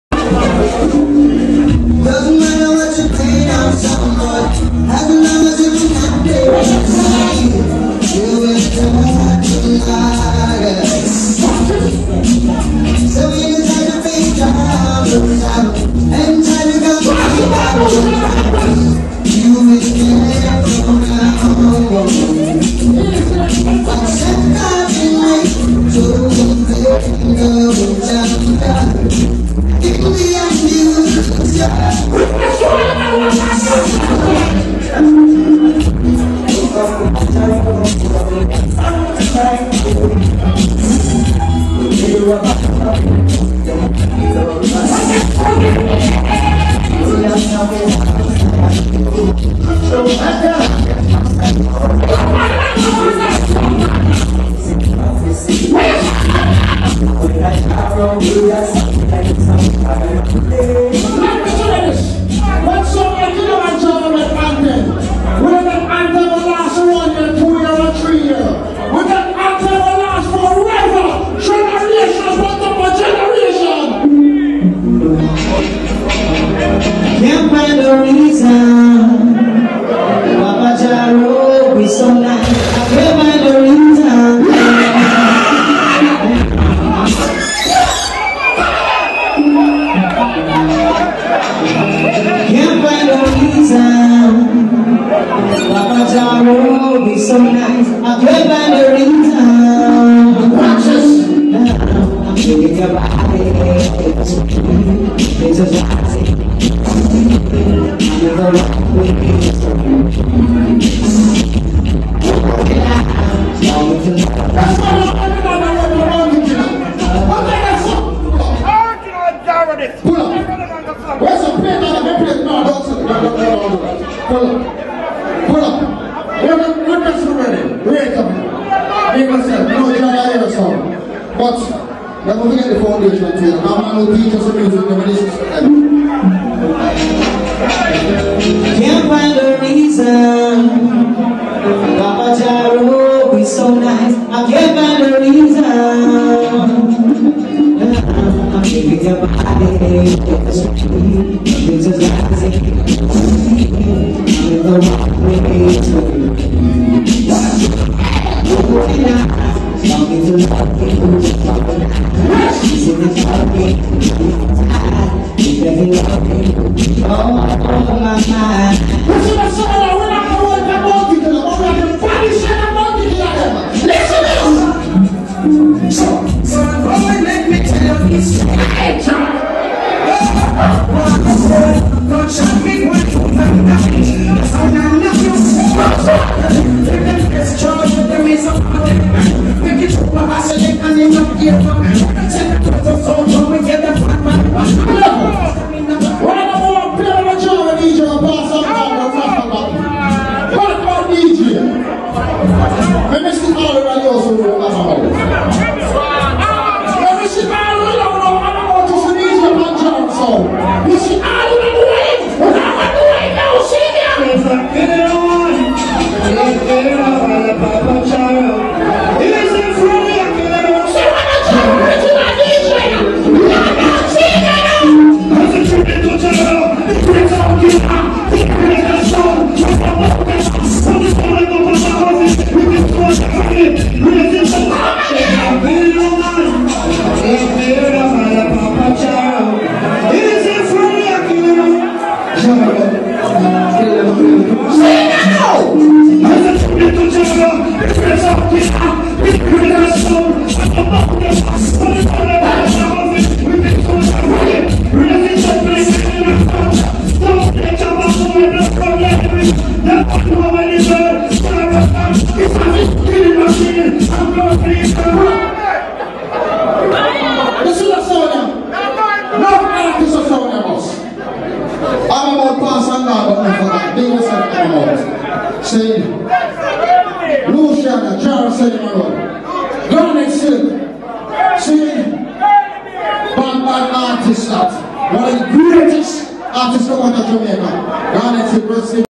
THE CAVENDISH RECORDED BY JOYE TAYLOR
SILK PROMOTIONS @ MR C PRESENTED THE BEST OF SIR DAVID RODIGAN
ALONGSIDE KILLAMAN JARO SATURDAY 14TH JUNE 2014 RECORDED BY JOYE TAYLOR